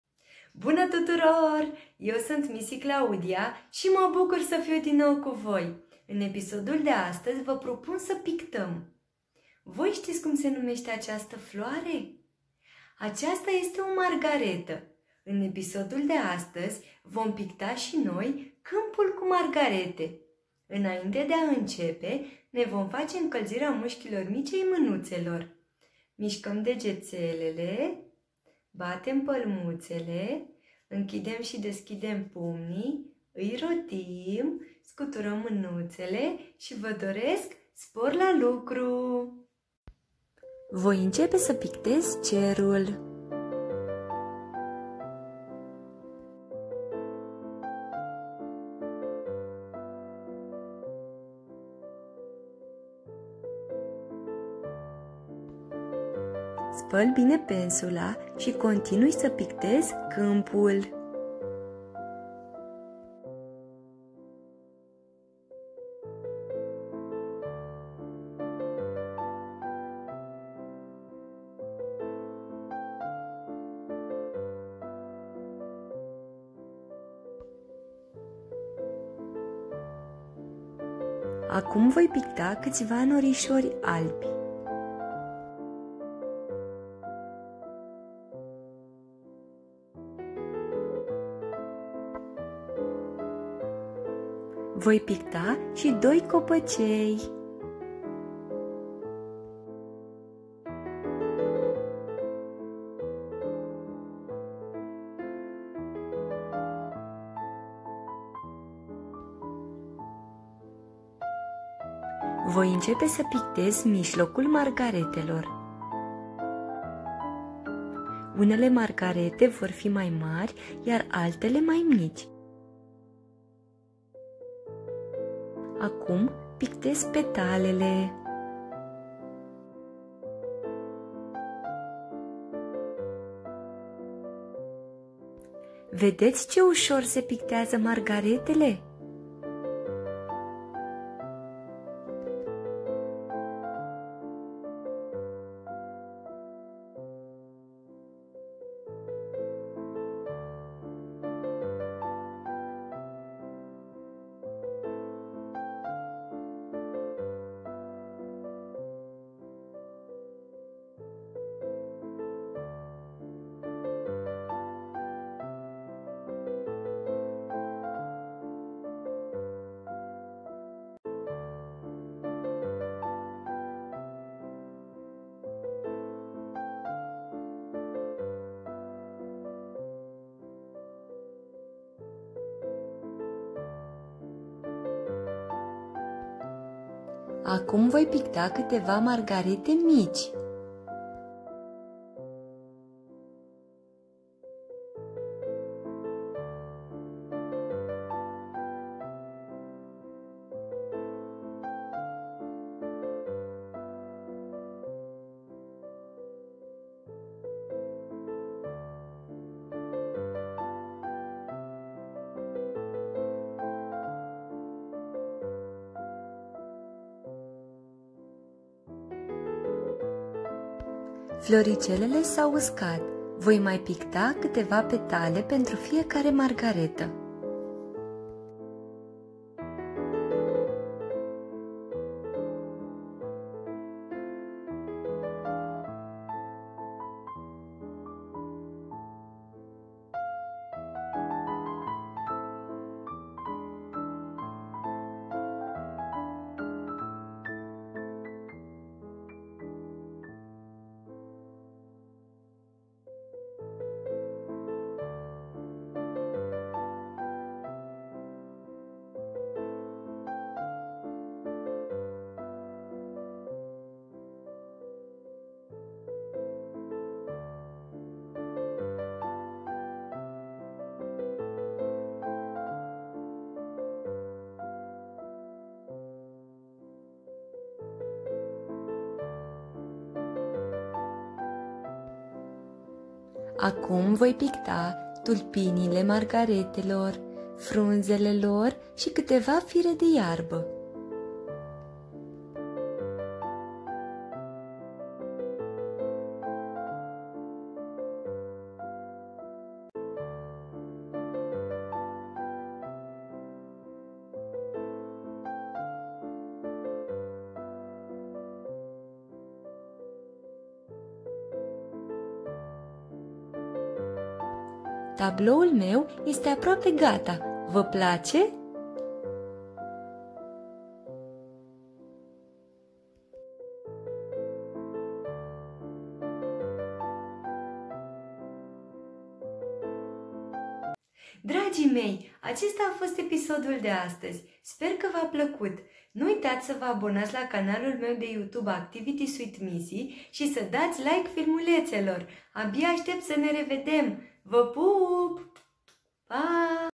Câmpul cu margarete.Pictură ușoară pentru copii
În acest filmuleț este prezentat cum puteti sa pictati foarte ușor margarete. Nu uitați să vă abonați gratuit la canalul meu de youtube pentru a vedea noutățile!